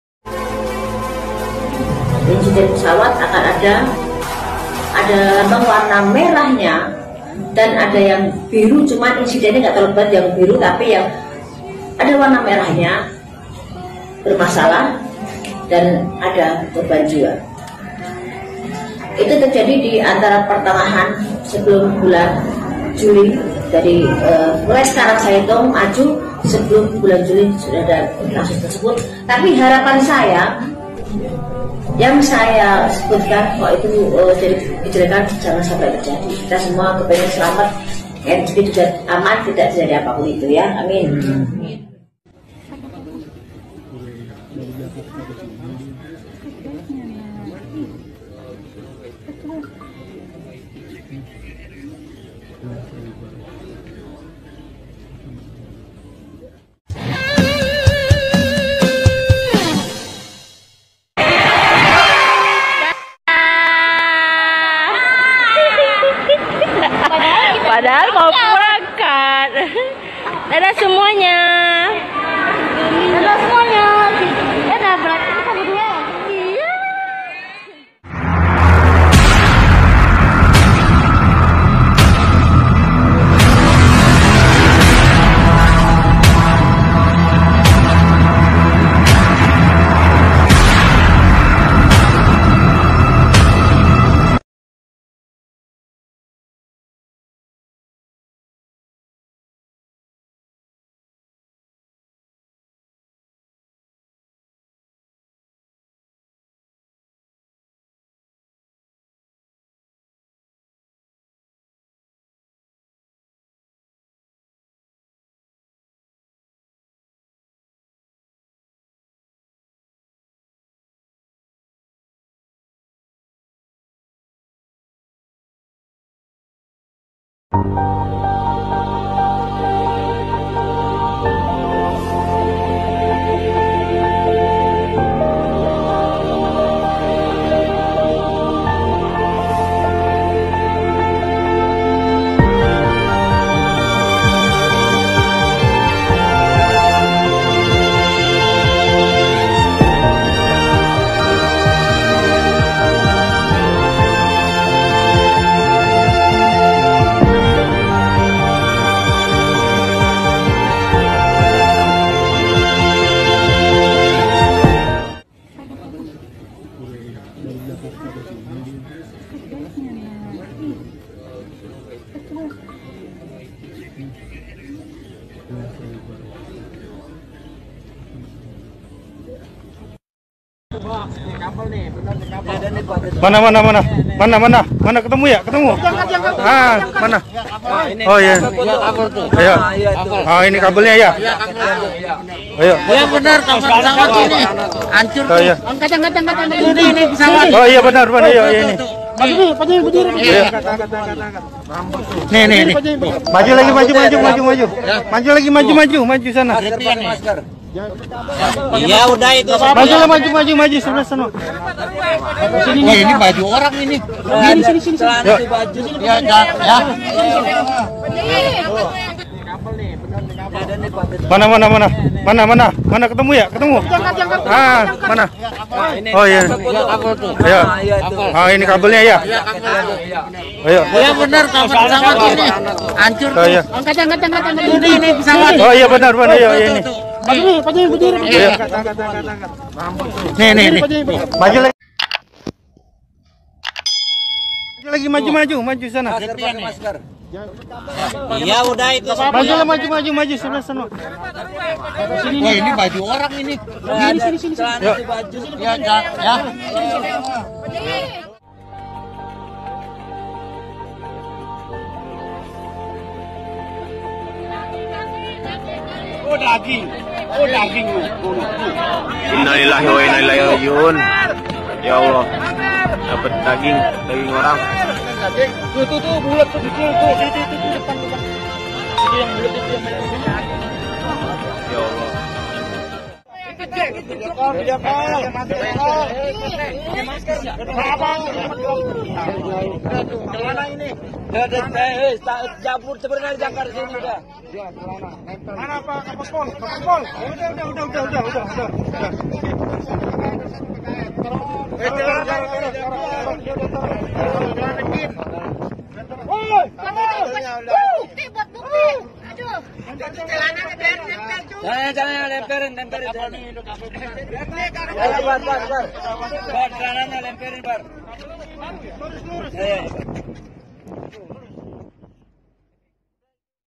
ini dia ramalan mbakyu pesawat jatuh di tahun 2021
innalillahiwainnailahirojiun indonesia berduka pesawat sriwijaya musibah jatu di perairan ternyata ini ramalanya mbakyu yuk simak vidionya